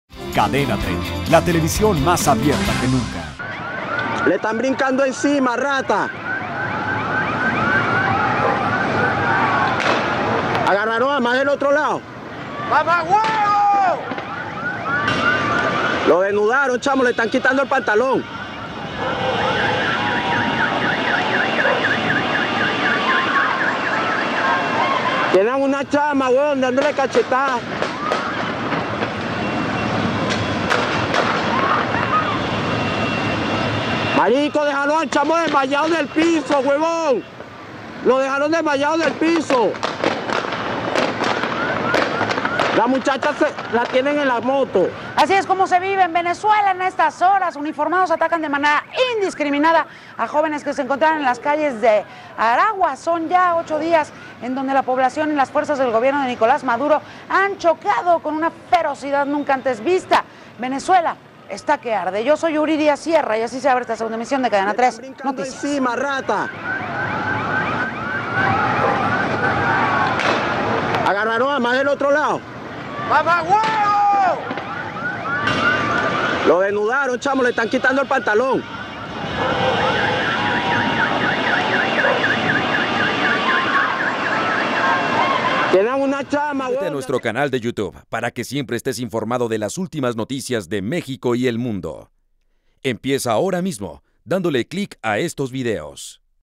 Sigue aumentando la tensión en Venezuela, uniformados atacan a los jóvenes
21 febrero 2014

Sigue aumentando la tensión en Venezuela mientras uniformados atacan de manera indiscriminada a los jóvenes.